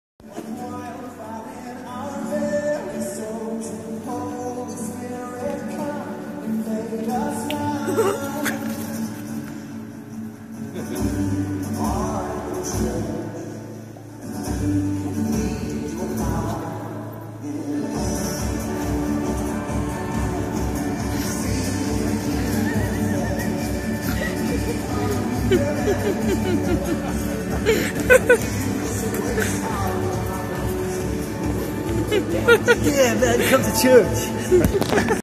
revmatt
The clergy team (rev matt, rev neal and youth worker, dan broom) showing us how to wind down and have fun with god!